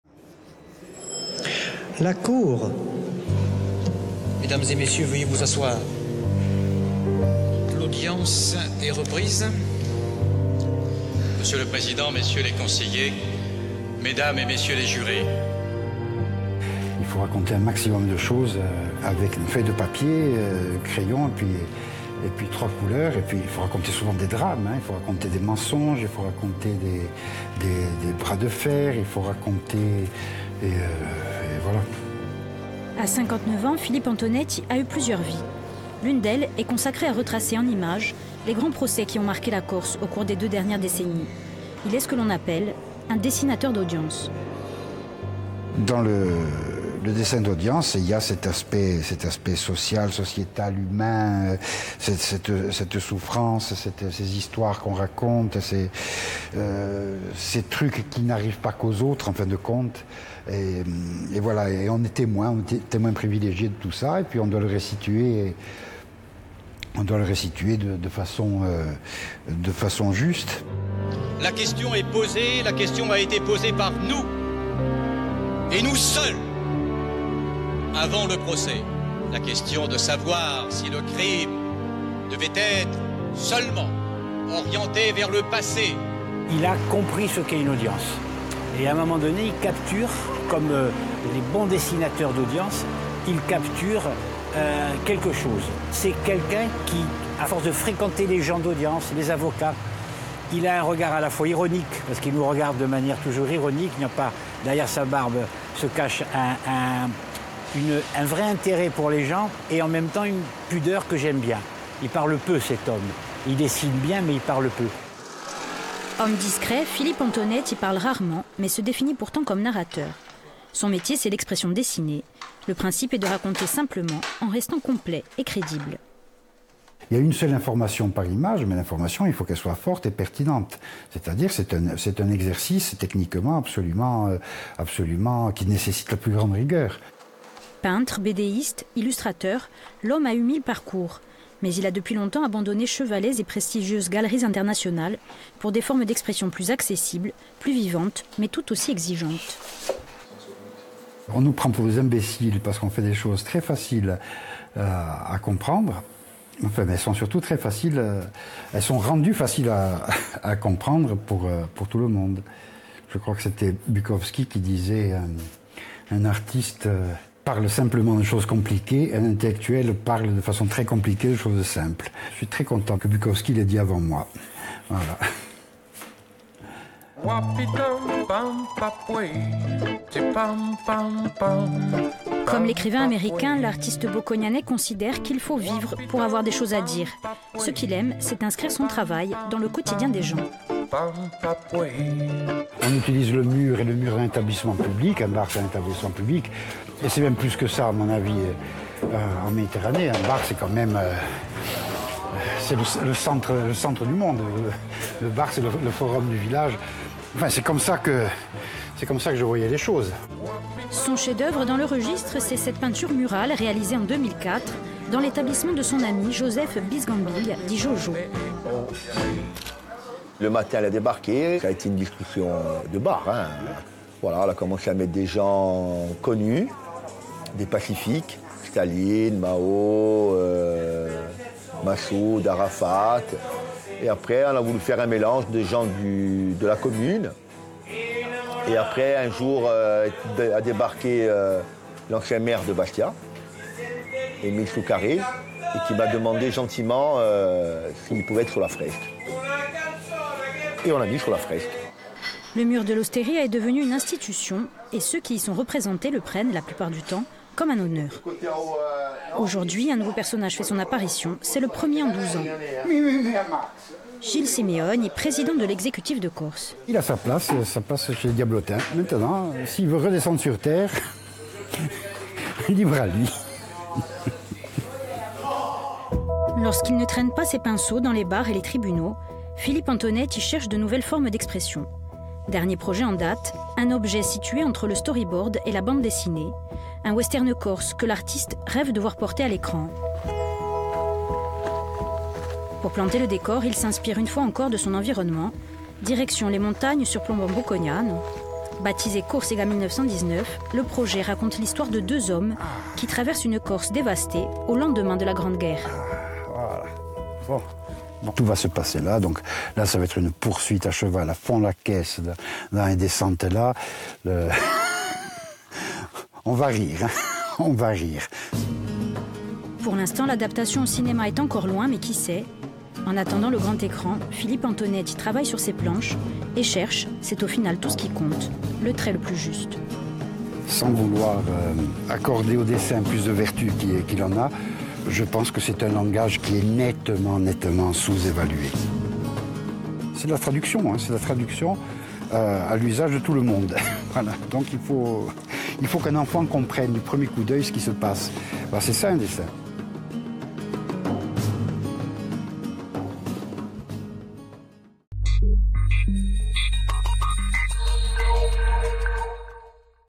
Portrait : Philippe Antonetti, artiste dessinateur d'audience
Intervenants : Philippe Antonetti - Me Philippe Gatti, avocat - Joseph Bisgambiglia, restaurateur

Equipe : SERRERI Kael - LUCIANI Lionel - GINESTE Christophe